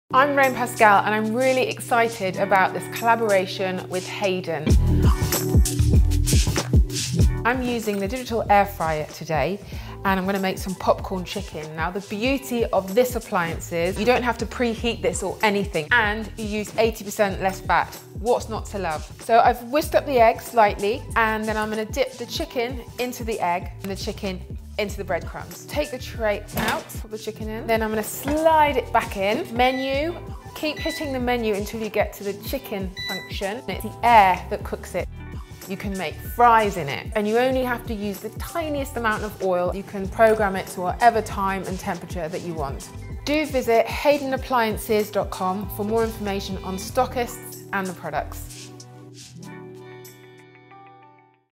Haden Black & Copper Digital Air Fryer
Black & Copper Digital Air Fryer.
Fry, bake roast and grill your favourite meals with a fraction of the oil.
A 60 minute timer with auto switch off for precise cooking.
Pre-set functions with a touchscreen control.
2.6 litre basket - easily serving a family of 4.
Adjustable thermostat - up to 200°c